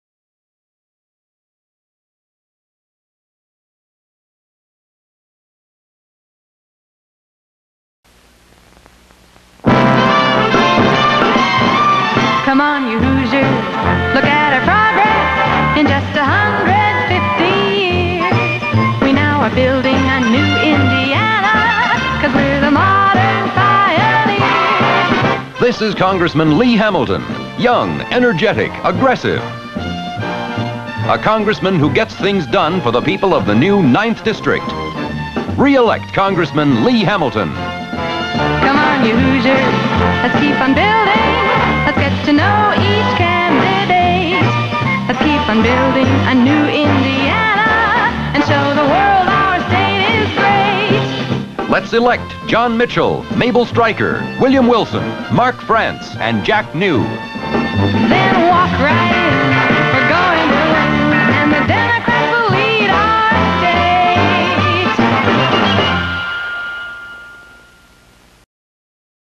Lee H. Hamilton [D-IN] 1966 Campaign Ad "State Ticket 2"
1966 United States House of Representatives Offices and Issue Campaign advertisement for Lee Herbert Hamilton [Democratic]. Photos of rec and industry in Indiana with a jingle and voiceover about electing officials who can continue progress.

Creator: not listed

Location: Indiana, United States

Format: 16mm film; video/mp4

Length: 1 minutes 10 seconds

Subjects: political campaign; Indiana elections

Contact The Carl Albert Congressional Research and Studies Center to purchase a non-watermarked high resolution digital file‪

Category
Education; Politics; Campaign Advertisements